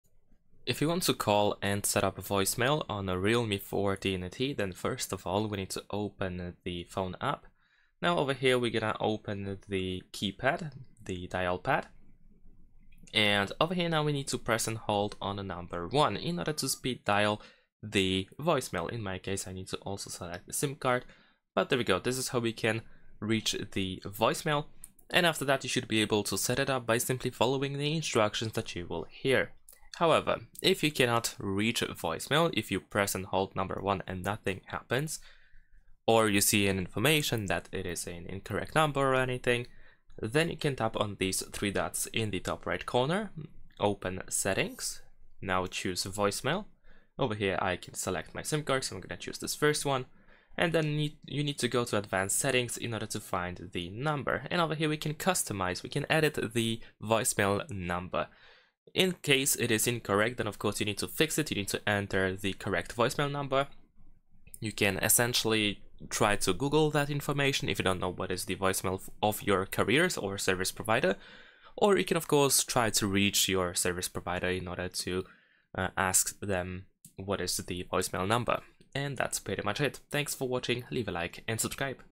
Realme 14T: How to Call & Set Up Voicemail
Want to set up voicemail on your Realme 14T? This complete guide shows you how to call your carrier's voicemail service and configure your mailbox. Learn the step-by-step process to record greetings, set a PIN, and customize notification preferences. We'll also show you how to access visual voicemail features and troubleshoot setup issues on your Realme 14T smartphone.